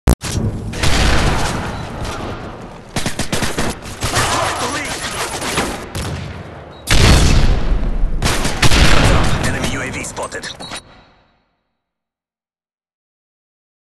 DJSNK - Spawnocide#1
Notice how I died...then spawned in the same location. Then this guy that was killed across the map spawns directly in front of me. All he had to do is pull the trigger. SMH